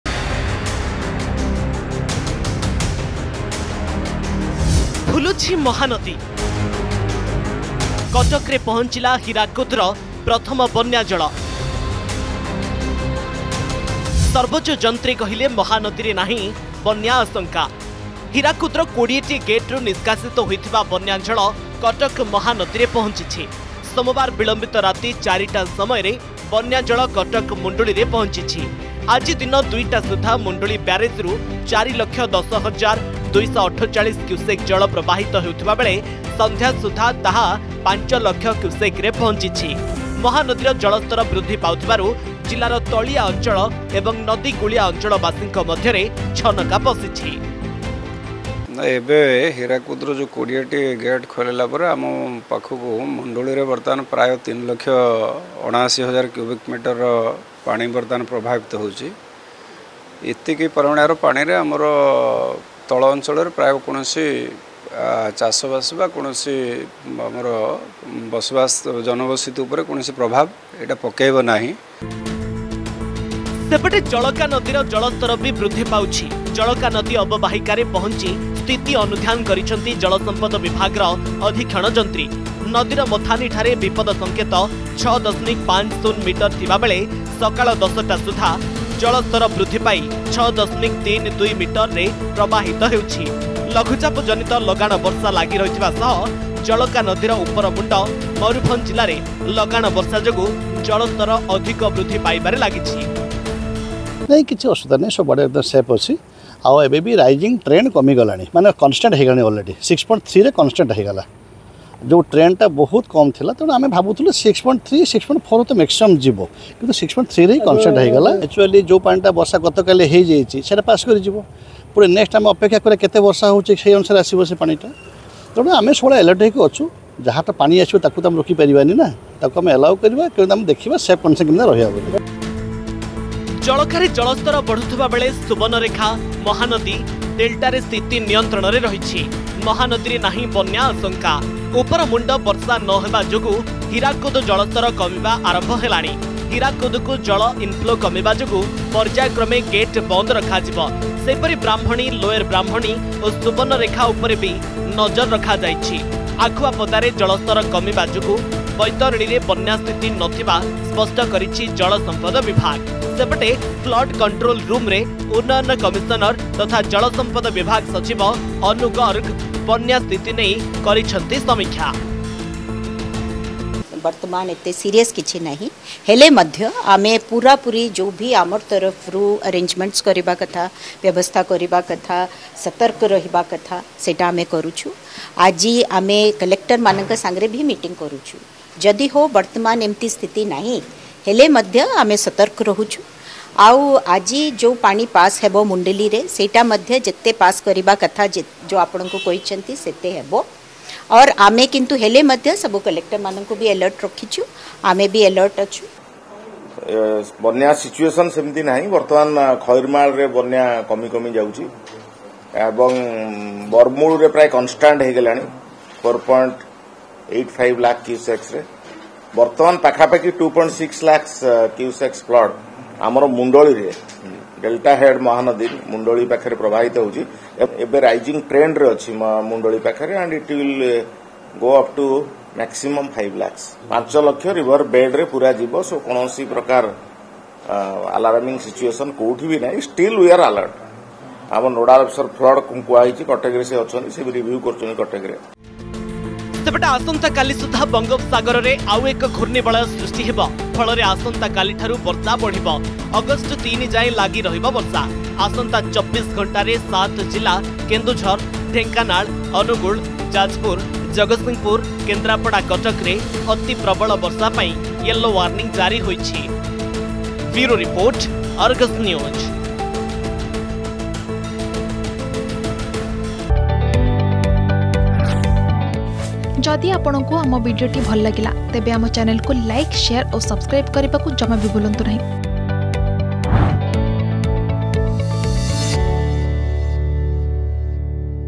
କଟକରେ ପହଞ୍ଚିଲା ହୀରାକୁଦର ପ୍ରଥମ ବନ୍ୟା ଜଳ || Flood || Hirakud
କଟକରେ ପହଞ୍ଚିଛି ହୀରାକୁଦର ପ୍ରଥମ ବନ୍ୟା ଜଳ । ଫଳରେ ଫୁଲିବାରେ ଲାଗିଛି ମହାନଦୀ । ଆତଙ୍କରେ ନଦୀ କୁଳିଆ ଓ ତଳିଆ ଅଞ୍ଚଳ ବାସିନ୍ଦା । ସର୍ବୋଚ୍ଚ ଯନ୍ତ୍ରୀ କହିଛନ୍ତି, ମହାନଦୀରେ ନାହିଁ ବନ୍ୟା ଆଶଙ୍କା ।